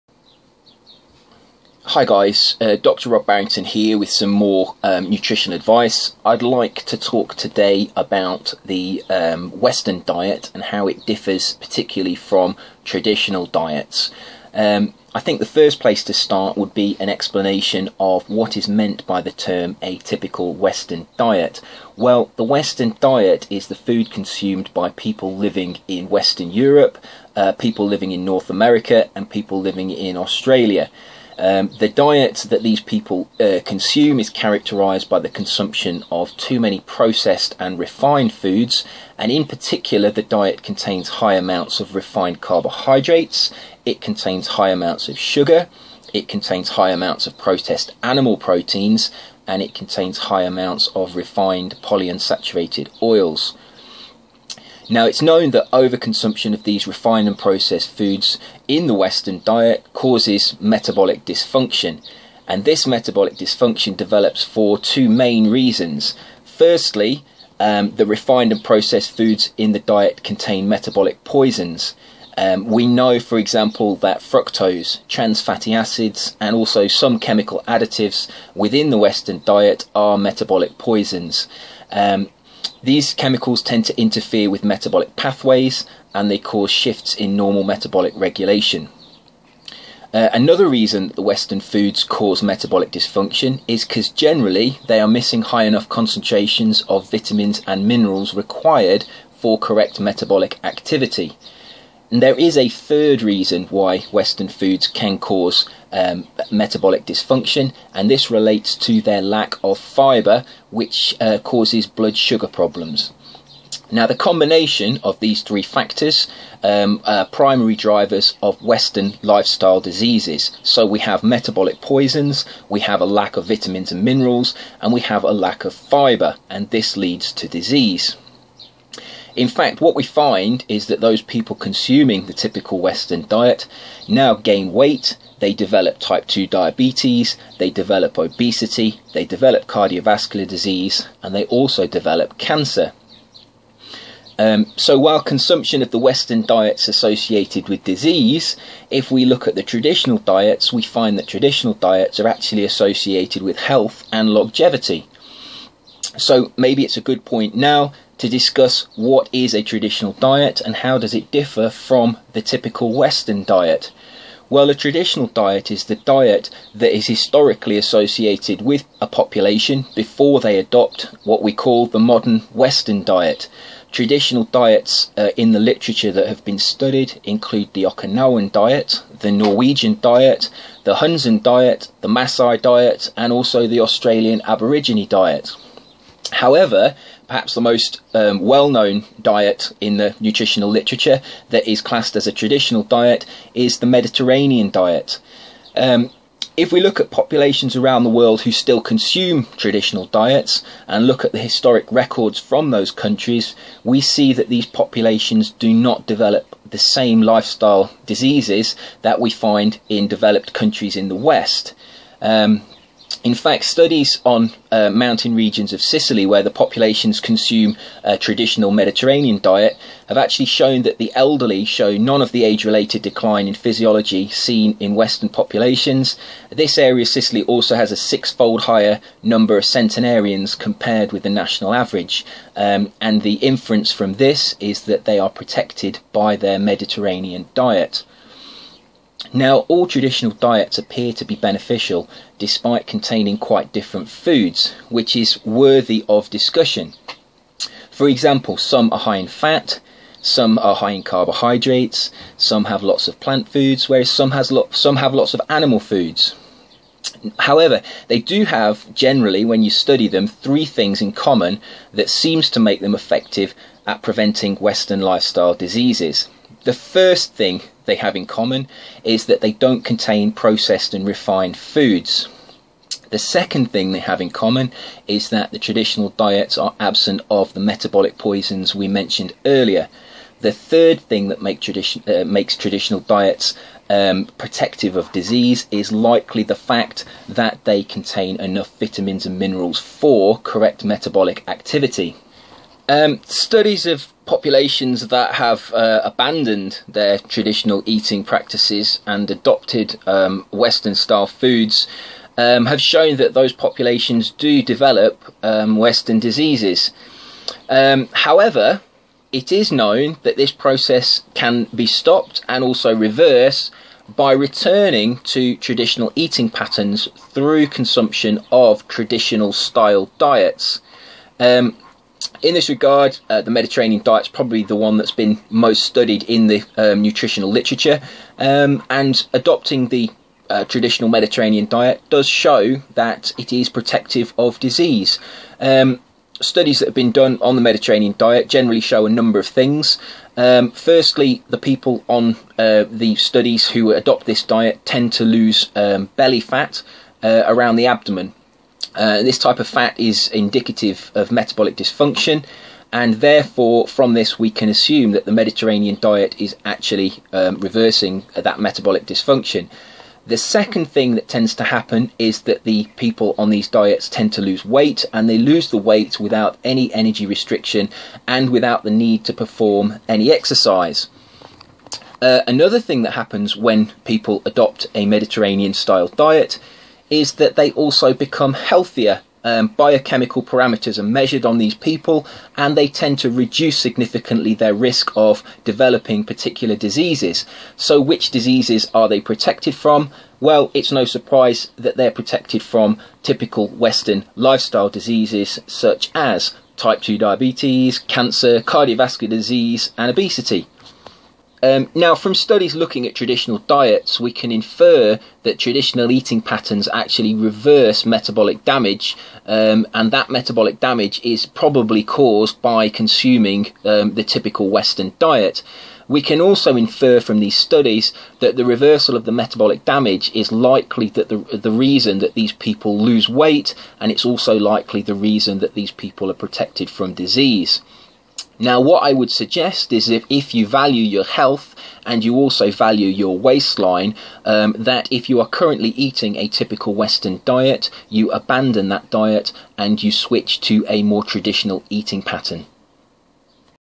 Traditional Diets
A short video on the reason why you should abandon a typical Western diet and adopt a more traditional eating pattern.